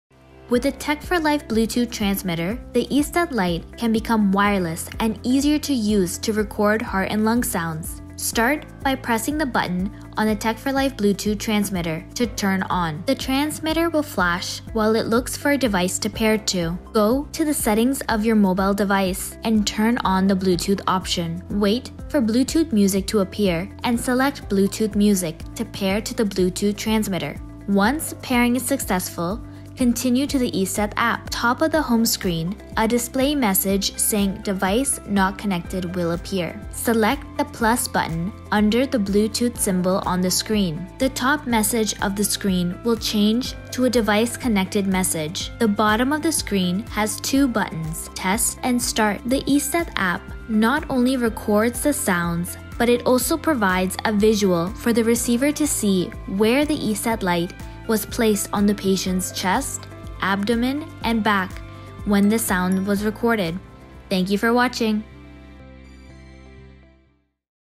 eSteth Lite-Digital demonstration with Bluetooth
eSteth Lite - Digital is a world-class digital stethoscope designed to provide high-quality heart and lung sounds to physicians and nurses to result in a better diagnosis. eSteth also provides state-of-the-art support for telemedicine. eSteth Lite is an affordable model, designed to support care providers in primary care and hospital settings.

*Sound amplification up to 100 times
*Sound recording with push of a button
*Patient sounds storage for future reference
*Ability to transfer sounds to a remote site during Teleconsultation